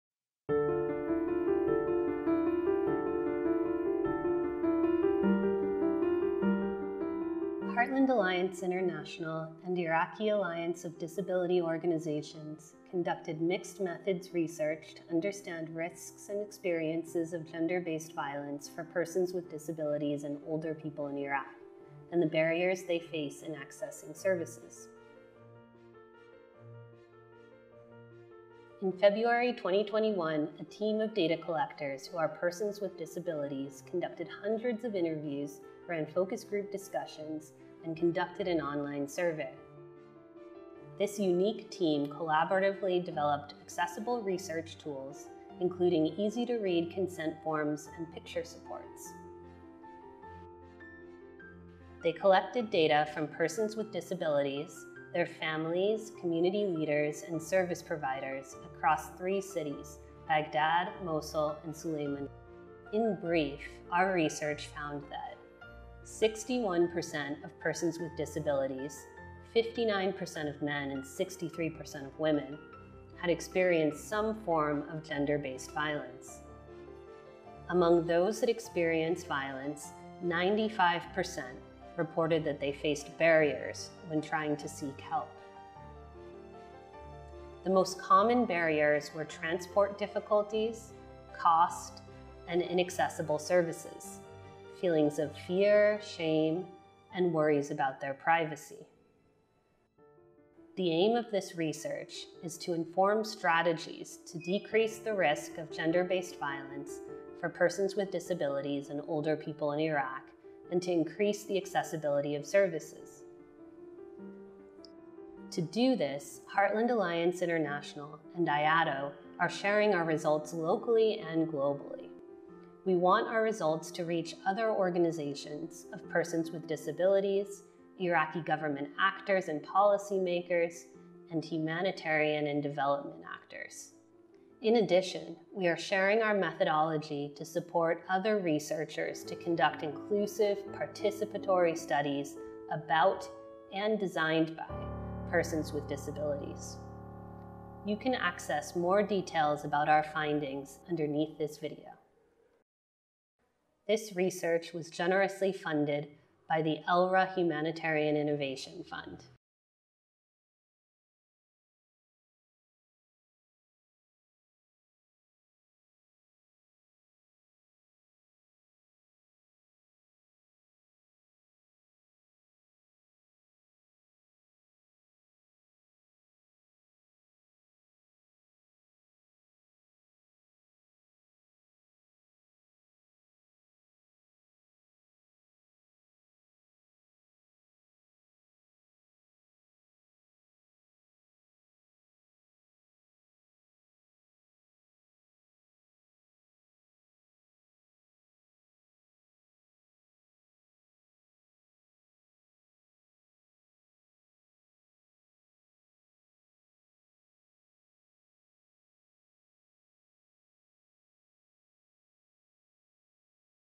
GBV & help-seeking among persons with disabilities in Iraq: Snapshot (Kurdish)
Heartland Alliance International (HAI) and Iraqi Alliance of Disability Organizations (IADO) conducted mixed methods research to assess experiences of gender-based violence (GBV) and barriers to accessing GBV service among persons with disabilities and older persons in Baghdad, Mosul, and Sulaymaniyah.